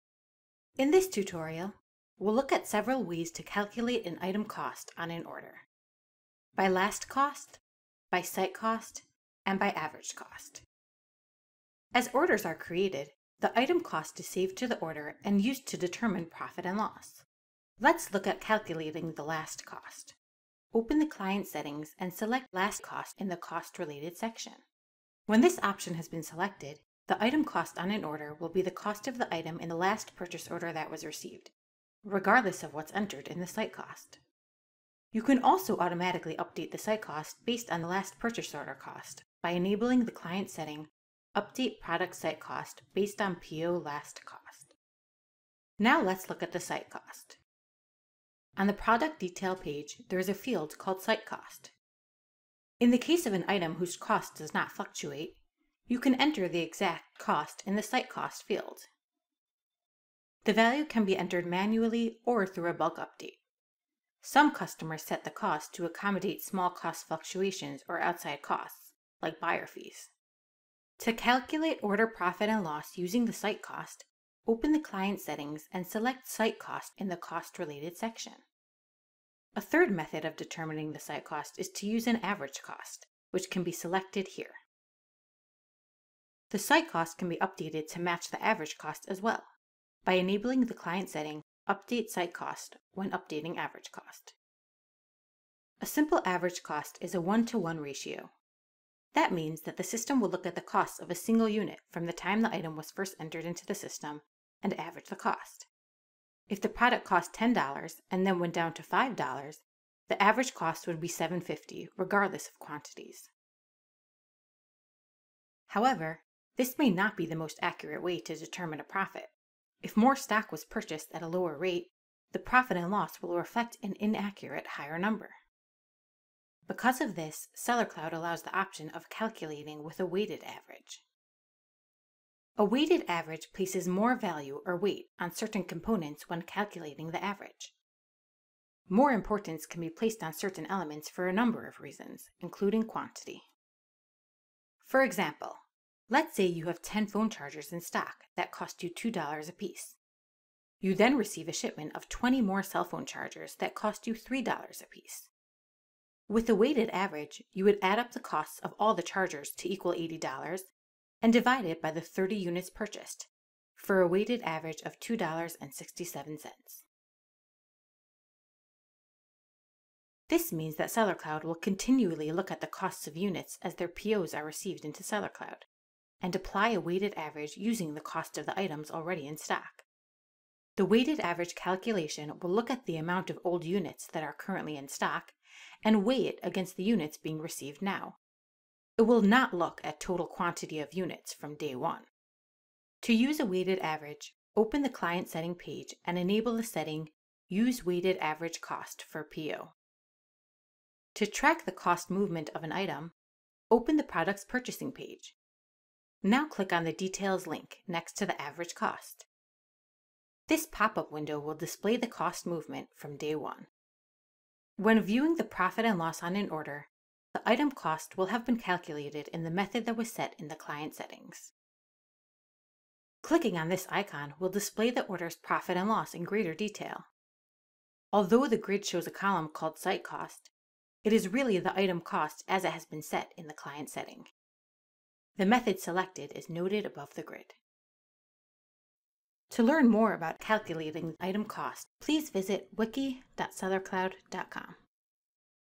Calculating Item Cost - SellerCloud - Order Management - 3.10
Learn how to calculate the cost of an item on a order with Last Cost, Site Cost, or Average Cost. Averages can be calculated simply or with a weighted average. (text of video below)

In this tutorial we'll look at several ways to calculate Item Cost on an order: By Last Cost, by Site Cost, and by Average Cost.
As orders are created, the item cost is saved to the order and used to determine profit and loss.
Let's look at calculating with the Last Cost.
To calculate order profit and loss using the Last Cost, open the Client Settings and select "Last Cost" in the Cost Related section.
When this option has been selected, the item cost on an order will be the cost of the item in the last purchase order that was received, regardless of what's entered in the site cost.
You can also automatically update the site cost based on the Last Purchase order cost by enabling the client setting "Update Product SiteCost based on PO LastCost"
Now let's look at the site cost.
On the Product detail page there is a field called Site Cost. In a case of an item whose cost does not fluctuate, you can enter the exact cost in the Site Cost field. The value can be entered manually or through a bulk update. Some customers set the cost to accommodate small cost fluctuations or outside costs, like buyer fees.
To calculate order profit and loss using the Site Cost, open the Client Settings and select "Site Cost" for that option in the Cost Related section.
A third method of determining the site cost is to use an Average Cost, which can be selected here.
The site cost can be updated to the average cost as well by enabling the client Setting "Update SiteCost when updating AverageCost."
A simple average cost is a one to one ratio. This means that the system will look at the costs of a single unit from the time the item was first entered into the system, and average the cost. If the product cost 10 dollars then went down to 5 dollars the average cost would be $7.50 regardless of quantities.
However, this may not be the most accurate way to determine a profit. For example, if more stock was purchased at a lower rate, the profit and loss will reflect an inaccurate higher number.
Because of this SellerCloud allows the option of calculating with a weighted average.
A weighted average places more value, or weight, on certain components when calculating the average. More importance can be placed on certain elements for a number of reasons, including quantity. For example, let's say you have 10 phone chargers in stock that cost you $2.00 a piece. You then receive a shipment of 20 more cell phone chargers that cost you $3.00 a piece. With a Weighted Average, you would add up the costs of all the chargers to equal $80 and divide it by the 30 units purchased, for a weighted average of  $2.67.
This means that SellerCloud will continually look at cost of units as their PO's are received into SellerCloud and apply a weighted average using the cost of the items already in stock. The weighted average calculation will only look at the amount of old units that are currently in stock and weigh it against the units being received now. It will not look at total quantity of units from day one.
To use a weighted average, enable the client setting "Use weighted average cost for PO"
To track the cost movement of an item open the product's Purchasing page. Now click on the Details link next to the average cost. This pop up window will display the cost movement from day one.
When viewing the Profit and loss on an order, the item cost will have been calculated in the method that was set in the client setting.
Clicking on this icon will display the order's Profit and Loss in greater detail. Although the grid shows a column called site cost, it is really the item cost as it has been set in the client setting . The method selected is noted above the grid.